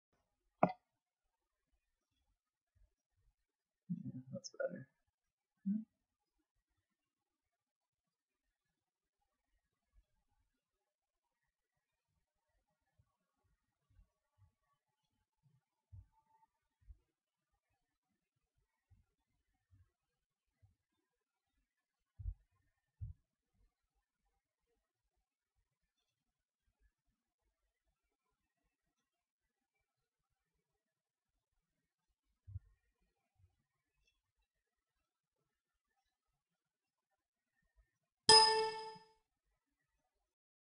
Target Tracking Using Double Pendulum - Double Motors
In this case we have two motors for LQR Control. One motor is at the base of the first arm and the second is at the joint of the first arm and second arm